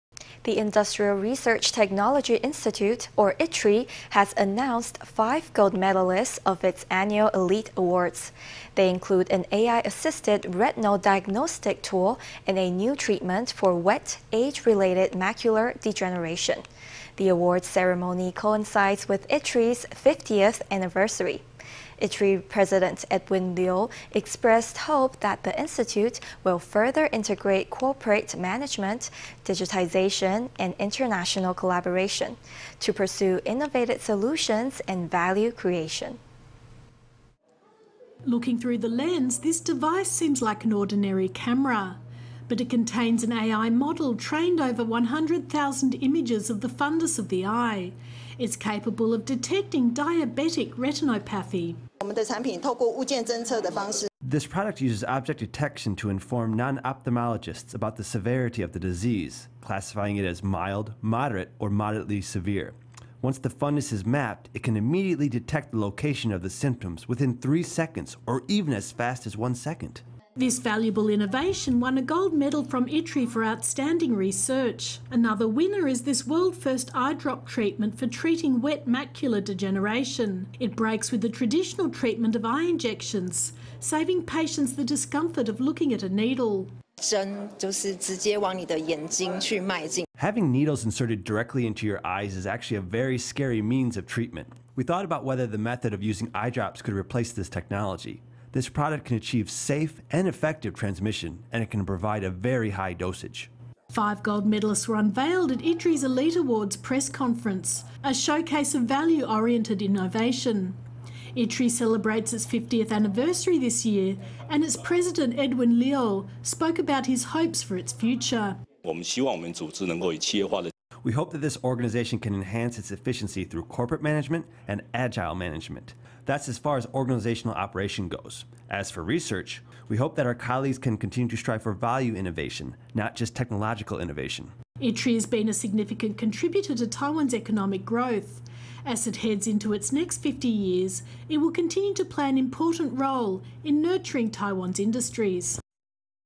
ITRI Elite Awards honor world-first eye drop treatment for wet macular degeneration
The Industrial Technology Research Institute, or ITRI, has announced five gold medalists of its annual Elite Awards. They include an AI-assisted retinal diagnostic tool and a new treatment for wet age-related macular degeneration. The awards ceremony coincides with ITRI’s 50th anniversary. ITRI President Edwin Liu expressed hope that the institute will further integrate corporate management, digitization, and international collaboration, to pursue innovative solutions and value creation.

Looking through the lens, this device seems like an ordinary camera. But it contains an AI model trained on over 100,000 images of the fundus of the eye. It’s capable of detecting diabetic retinopathy.

Lee Ya-wen
Industrial Technology Research Institute
This product uses object detection to inform non-ophthalmologists about the severity of the disease, classifying it as mild, moderate, or moderately severe. Once the fundus is mapped, it can immediately detect the location of the symptoms within three seconds or even as fast as one second.

This valuable innovation won a gold medal from ITRI for outstanding research. Another winner is this world-first eye drop treatment for treating wet macular degeneration. It breaks with the traditional treatment of eye injections, saving patients the discomfort of looking at a needle.

Cheng Shu-chen
Having needles inserted directly into your eyes is actually a very scary means of treatment. We thought about whether the method of using eye drops could replace this technology. This product can achieve safe and effective transmission, and it can provide a very high dosage.

Five gold medalists were unveiled at ITRI’s Elite Awards press conference, a showcase of value-oriented innovation. ITRI celebrates its 50th anniversary this year, and its president, Edwin Liu, spoke about his hopes for its future.

Edwin Liu
Industrial Technology Research Institute president
We hope that this organization can enhance its efficiency through corporate management and agile management. That’s as far as organizational operation goes. As for research, we hope that our colleagues can continue to strive for value innovation, not just technological innovation.

ITRI has been a significant contributor to Taiwan’s economic growth. As it heads into its next 50 years, it will continue to play an important role in nurturing Taiwan’s industries.